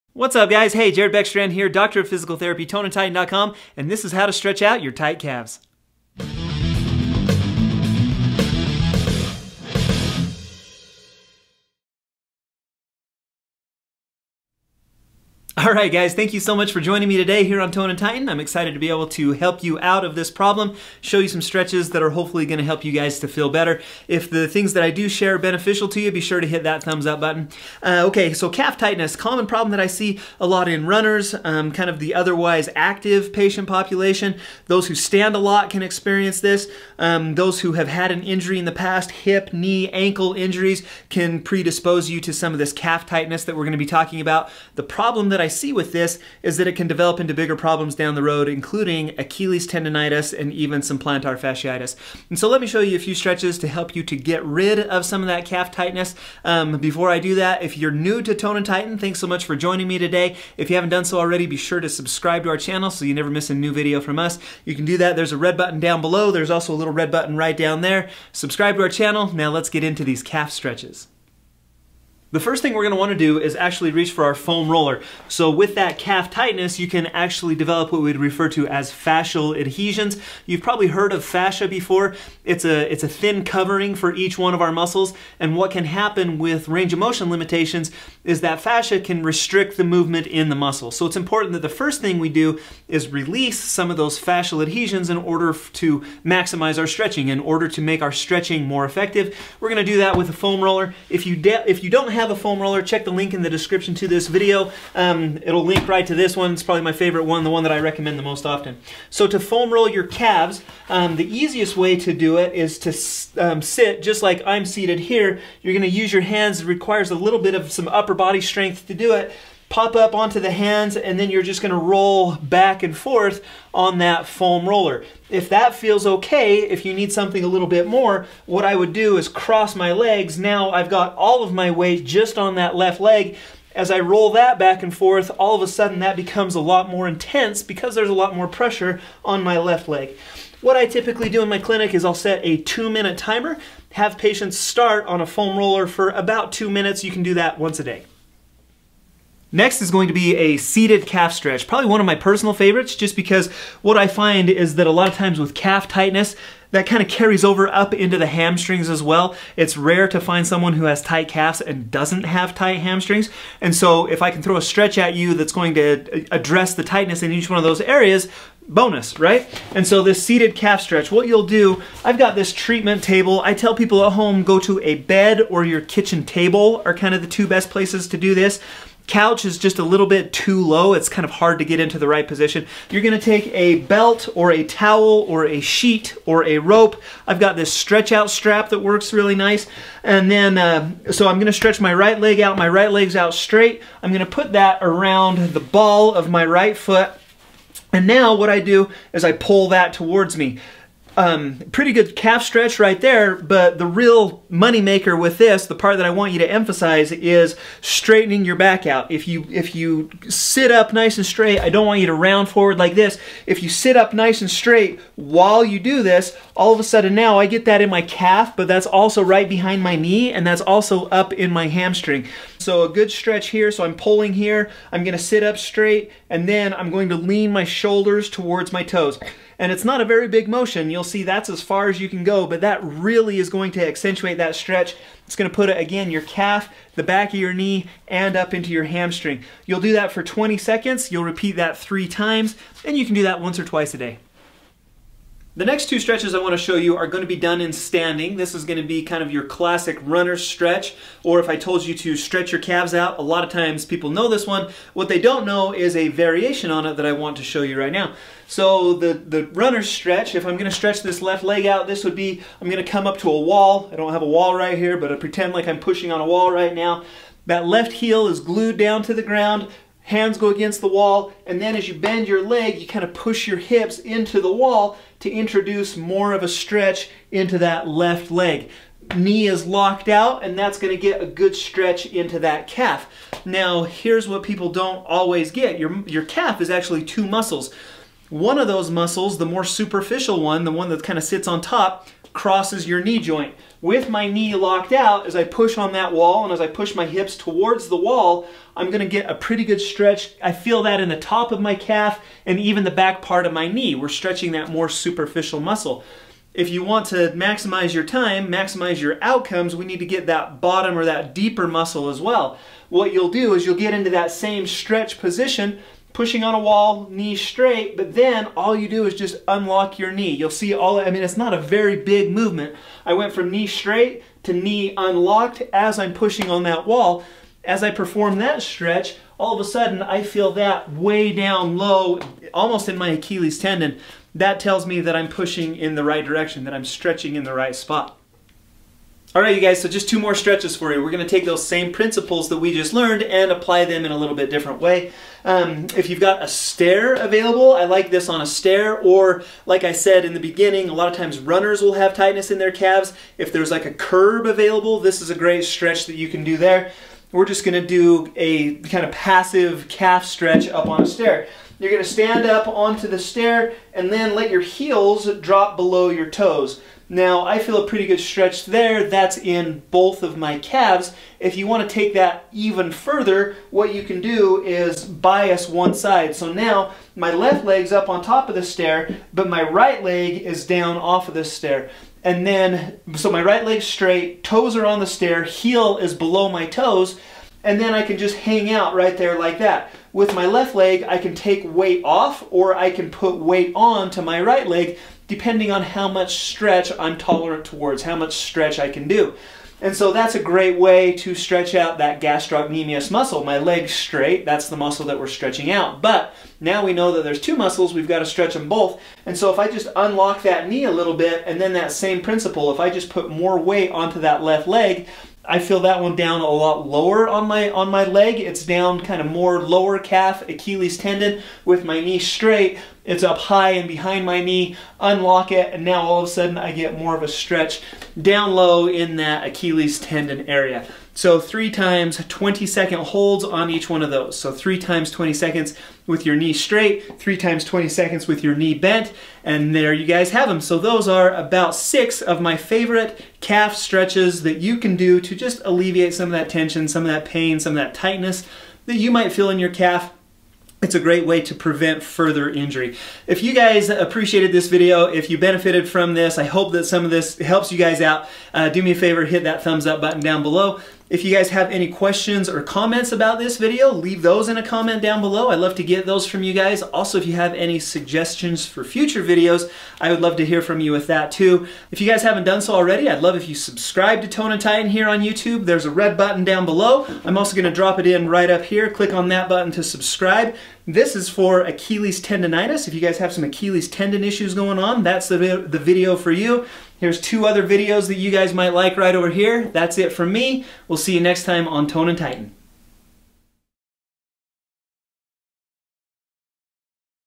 STOP Calf Pain | Best Stretches For Calves
6 of the best stretches for tight calves! How to stretch your calf muscles the right way as demonstrated by a physical therapist. Whether you have tight calves, a calf injury, or just need more mobility - these are the best exercises for calf pain and tightness.

Calf tightness is a common problem experienced by many people. Because the ankle joint is constantly working (whether walking, running, or standing, etc) the calves are typically under constant work.

I see this calf tightness and these calf injuries most often in runners and the running population, but none of us are completely immune from it.

The biggest problems with calf tightness is that it puts you at higher risk for a calf strain or injury, causes increased pull on your Achilles tendon (predisposing you for Achilles tendonitis), and also puts more stress on your plantar fascia (which can cause foot pain).

The best thing you can do for your tight calves is get on a regular schedule of stretching and other exercises that will ultimately help you alleviate this tension. The exercise demonstrated in this video are just the answers you're looking for.

The thing that I've found in working with hundreds of patients and clients who suffer from this problem is that not everyone knows how to stretch their calves out. This has to be done the right way in order to maximize your outcomes and results.

Did you know your "calf" is actually made up of two muscles? One sits right on top of the other and they both perform a similar function (plantar flexion at the ankle - like stepping on the gas). However they're very different anatomically and need to be stretched as such.

The GASTROCNEMIUS muscle is the more superficial (sits on top) of the two muscles. It actually crosses the knee joint and attaches up to the bottom of the femur and then inserts into the top part of your heel. This muscle is stretched with our knee extended as we pull our toes towards our shins.

The SOLEUS muscle sits underneath the gastrocnemius and up against the tibia. It attaches below the knee joint into the tibia and inserts into the top of our heel. You can actually best stretch the soleus by "unlocking" your gastrocnemius and bending your knee slightly. With your knee slightly bent you then pull your toes towards your shin until a stretch is felt in the back of your leg.

LIST OF STRETCHES DEMONSTRATED IN THIS VIDEO

1. FOAM ROLLER: great way to release the tightness and fascia that surround the calf muscles preparatory to stretching them out.

2. SEATED CALF STRETCH: bonus for those of you who also have tight hamstrings.

3. STANDING GASTROCNEMIUS STRETCH: push on the wall with your knee locked out to feel a stretch back behind your knee.

4. STANDING SOLEUS STRETCH: unlock the knee slightly to feel a much deeper stretch a lot lower towards your heel.

5. STANDING STAIR STRETCH (GASTROC): stand on the edge of a stair so your ankles fall below your toes.

6. STANDING STAIR STRETCH (SOLEUS): unlock your knee slightly to feel a comfortable stretch in your Achilles area.

I hope the principles and exercises I have demonstrated in this video help you to find some relief from some of the pain or discomfort you may have been experiencing.

👍🏼 Please show some love by hitting the "thumbs up" button to LIKE this video.